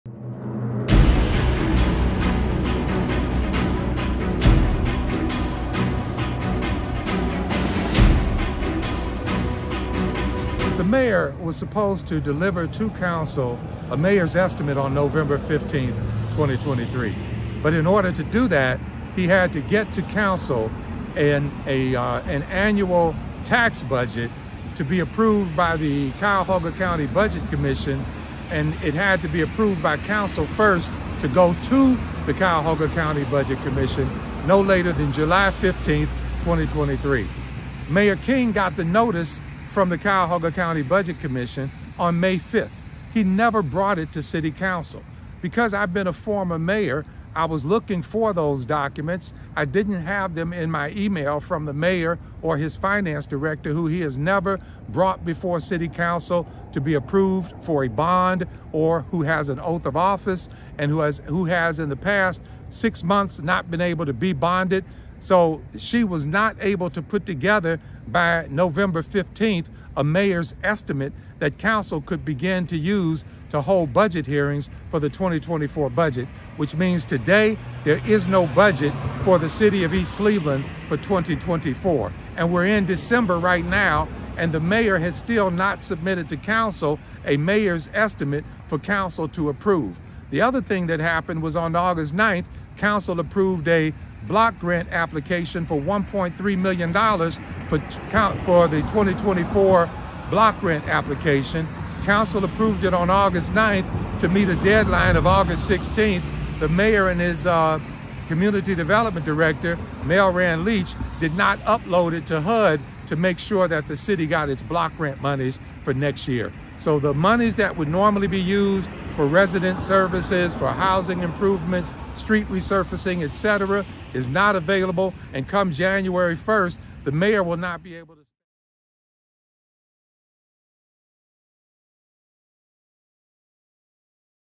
2023 East Cleveland Recall of Mayor #4
Giving away Forest Hill Park without reading the contract.